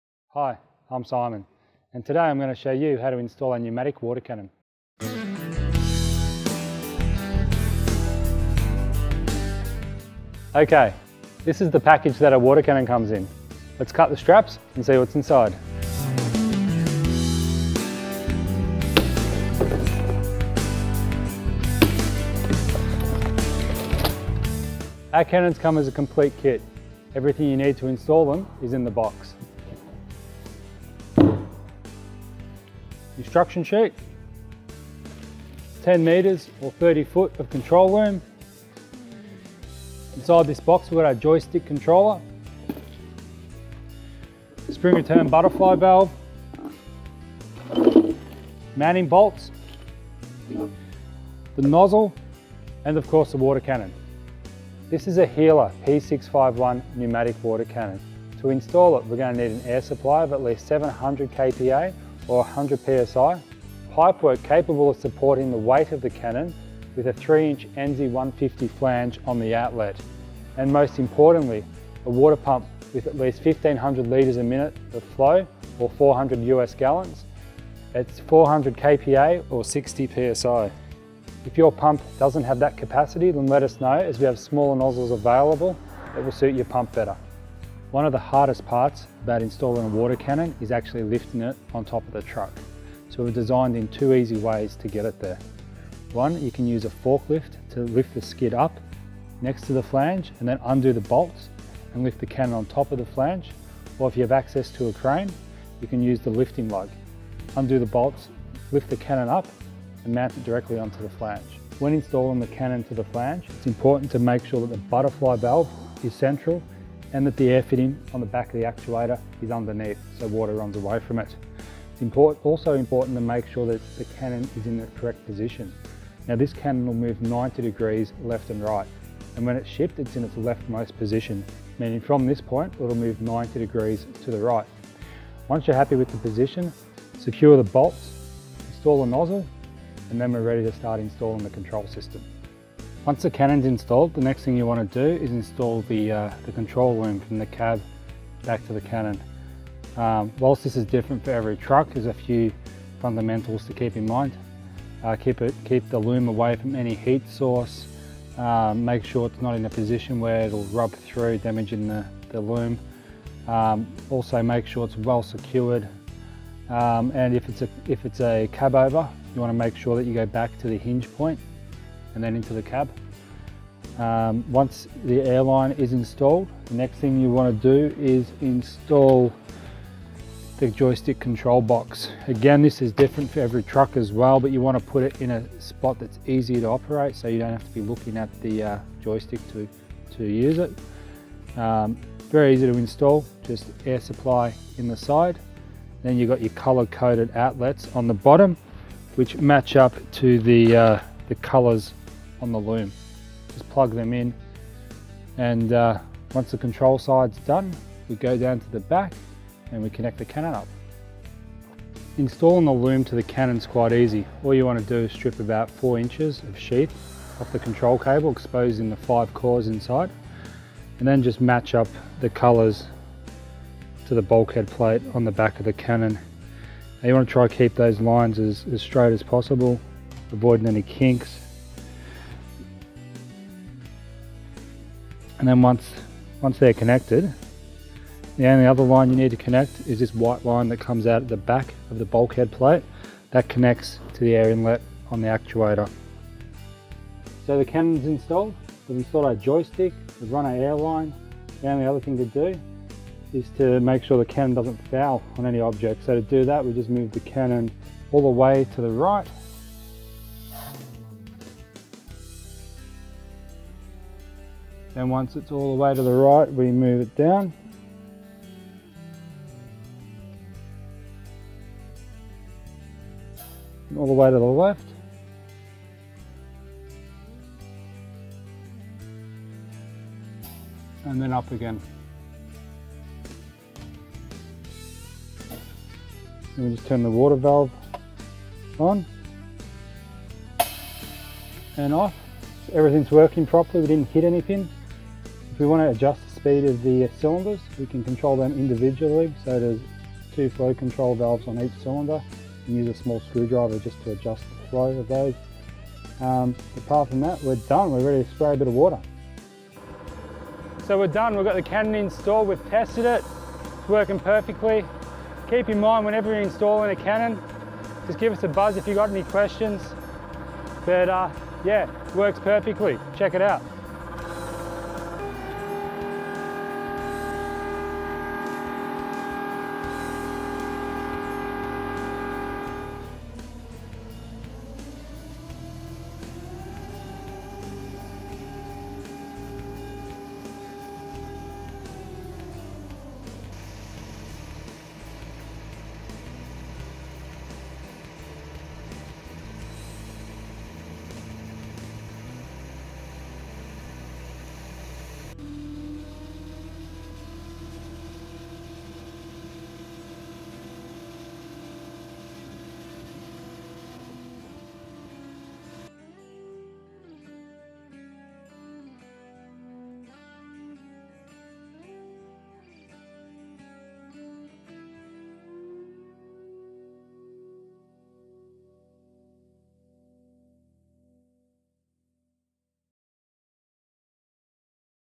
P651 Installation
Step by step guide on installing a P651 Pneumatic Water Cannon.

The P651 is available for purchase from the following companies.

Australia: Water Wise Water Trucks Australia.